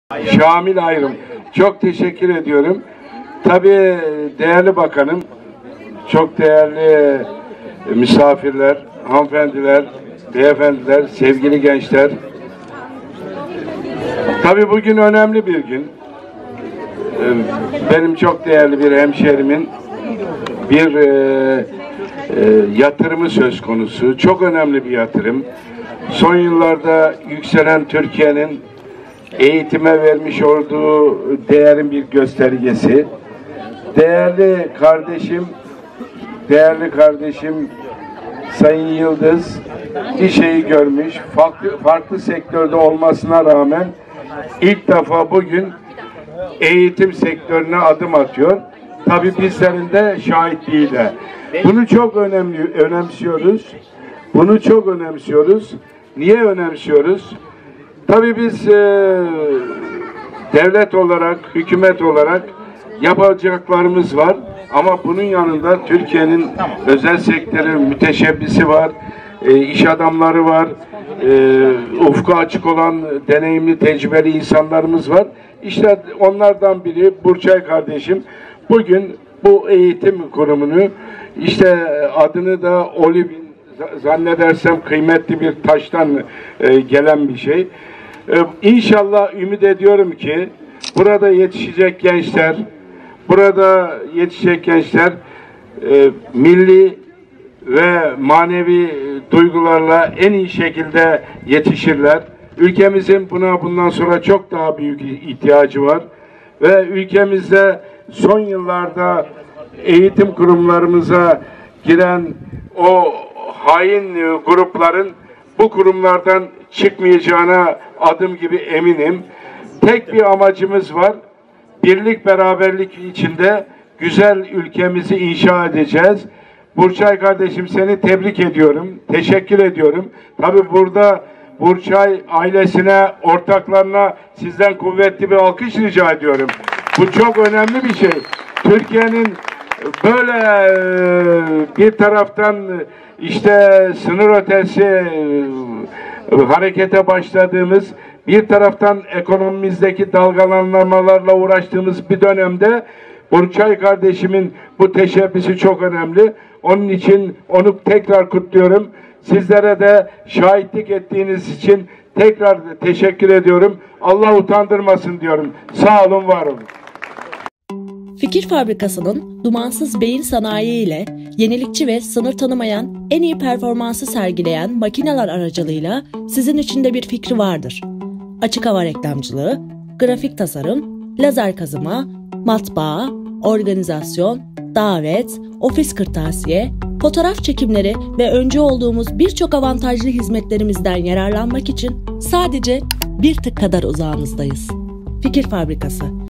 Zor Süreçten Geçiyoruz! Olivin Akademisi - Şamil Ayrım - start tv
Olivin Çocuk Akademisi Açılışını Eski bakan Ak Parti Kars Milletvekili Ahmet Arslan ve Ak Parti İstanbul Milletvekili Şamil Ayrım birlikte yaptı.

Olivin Çocuk Akademisi yönetim kurulu başkanı siyasetçi işadamı Burçay Yıldız, katılımcılara teşekkür etti.